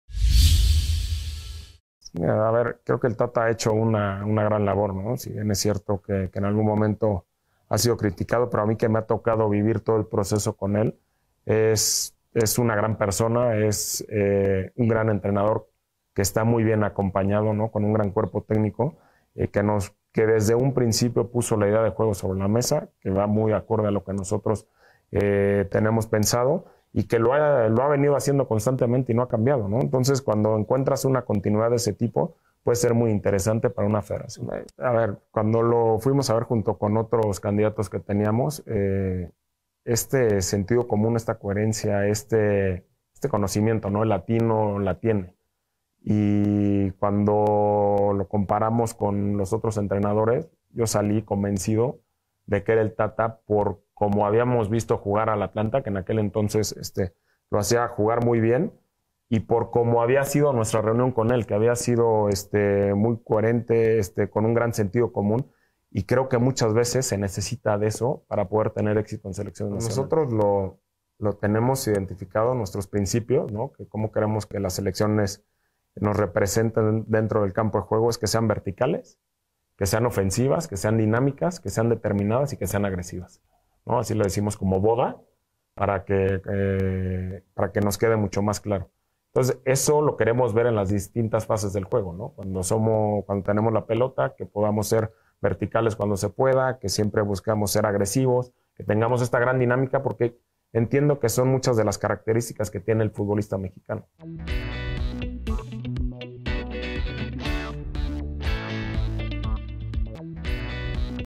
Torrado: "El Tata ha hecho una gran labor, puede ser muy interesante su continuidad"
En la Federación Mexicana de Futbol no ven con malos ojos que el "Tata" Martino se mantenga al frente de la Selección Nacional